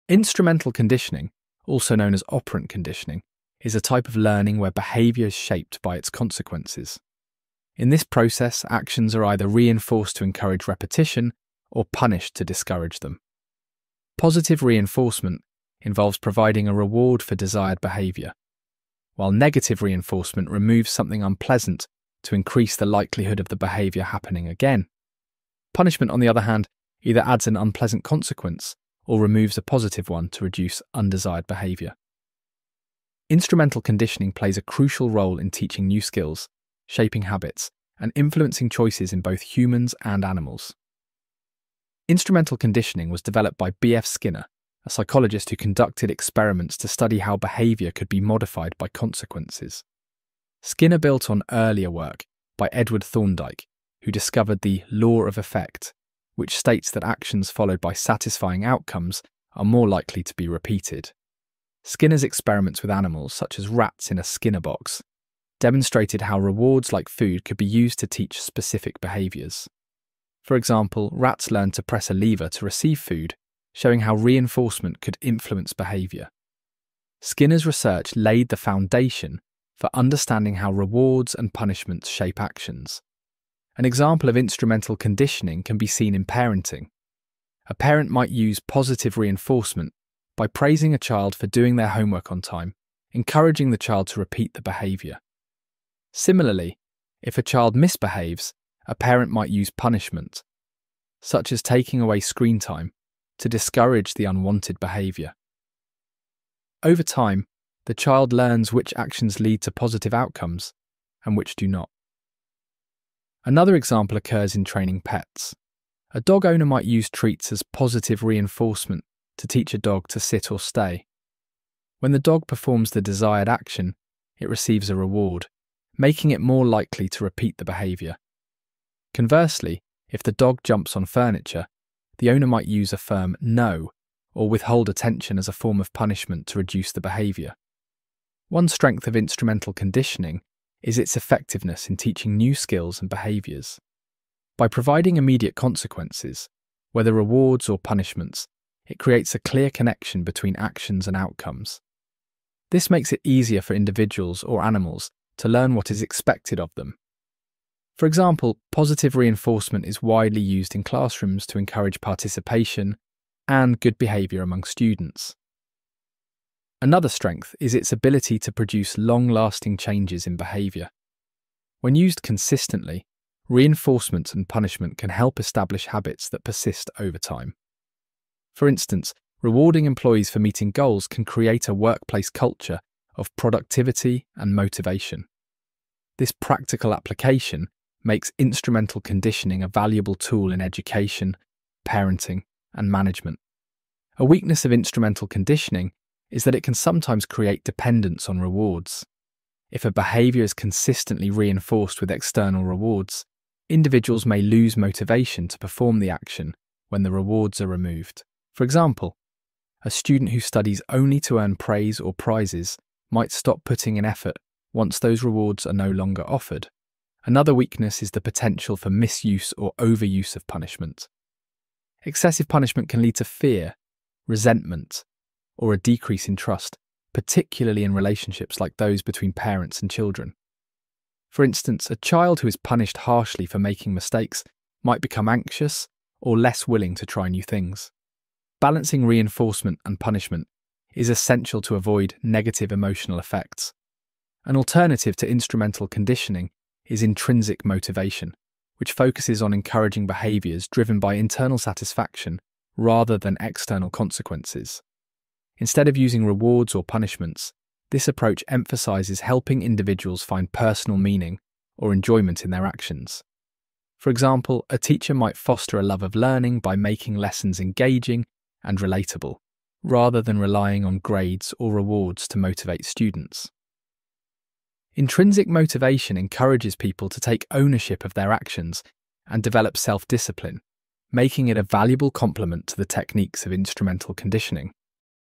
Instrumental Conditioning: Definition & Examples (5 Minute Explanation)
Instrumental conditioning, also known as operant conditioning, is a learning process in which behavior is influenced by the consequences that follow it. This concept, pioneered by psychologist B.F. Skinner, highlights how actions that are reinforced are more likely to occur, while those that are punished are less likely to be repeated. Reinforcement can be positive, such as receiving a reward for desired behavior, or negative, where an unpleasant stimulus is removed. Similarly, punishment can be used to decrease undesirable behaviors, either by introducing an adverse outcome (positive punishment) or removing a favorable condition (negative punishment).

Instrumental conditioning is widely used in various settings, from parenting to workplace management and animal training. For example, rewarding a child with praise for completing homework encourages similar behavior in the future, while grounding a teenager for skipping chores may deter rule-breaking. The process relies on understanding the relationship between behavior and its consequences, making it a powerful tool for shaping and modifying actions.